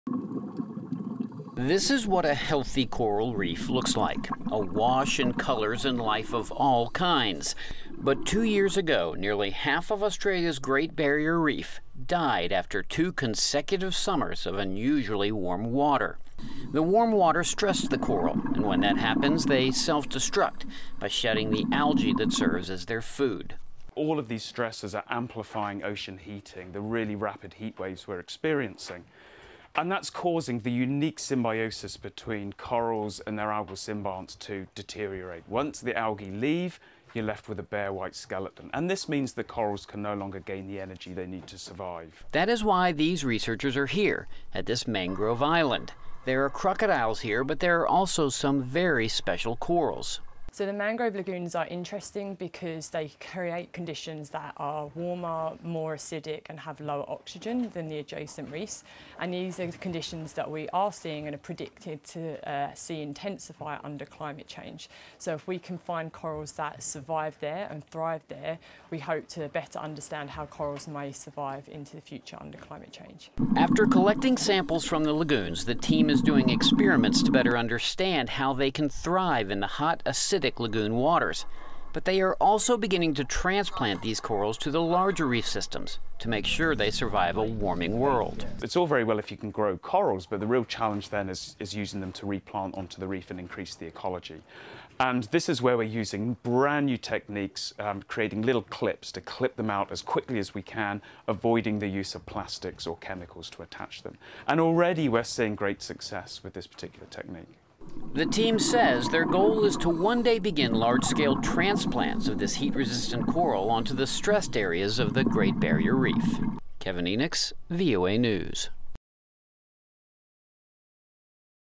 Searching for Coral That's Resistant to Climate Change
The world's coral reefs host an astounding amount of the ocean's biodiversity, but warming waters are putting them at risk around the world. Now a group of Australian scientists is searching for corals that can thrive in warm, acidic waters. VOA's Kevin Enochs reports.